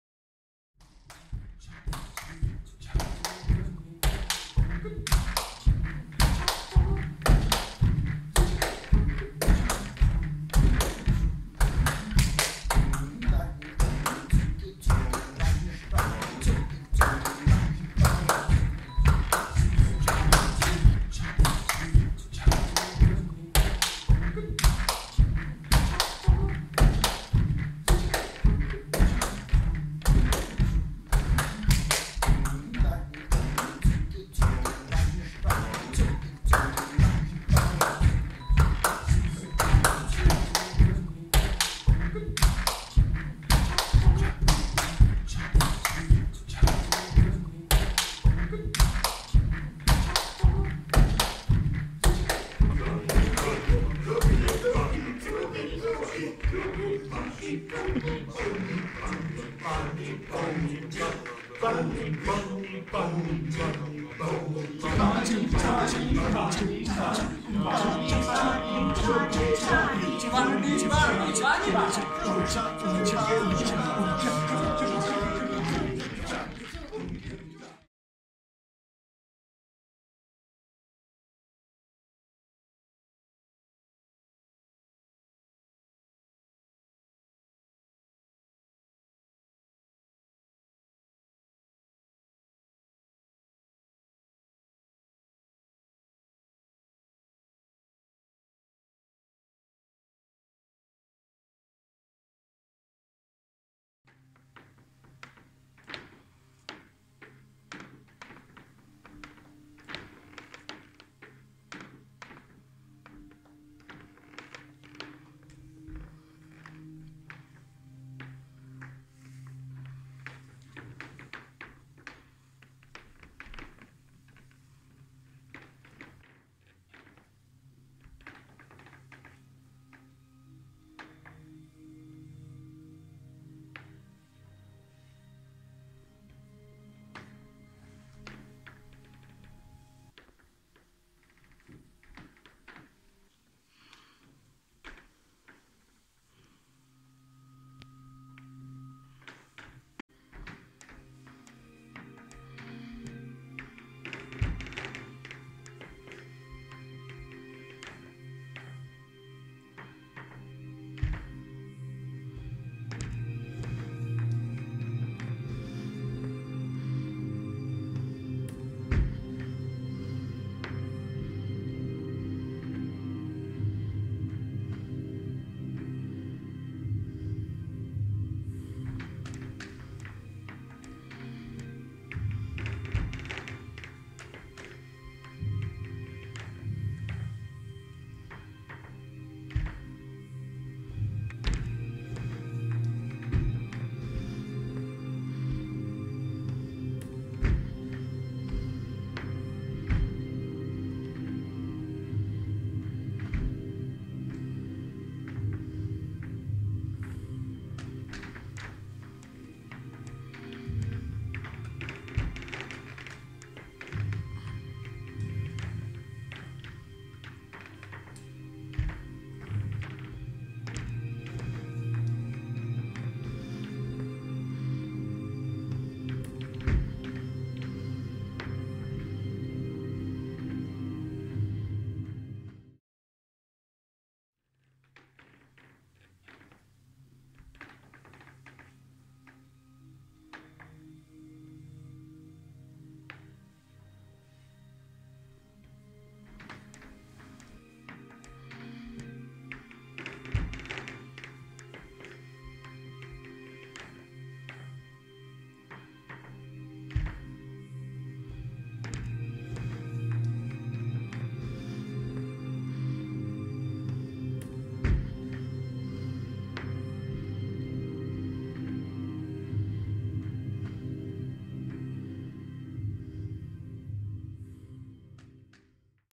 Teaching Each Other Lab
Impressions from B&W's first Teaching Each Other Lab, December 2018 @ Forsøgsstationen, Copenhagen, Denmark. An experiment in non-hierarchical teaching and learning in the performing arts.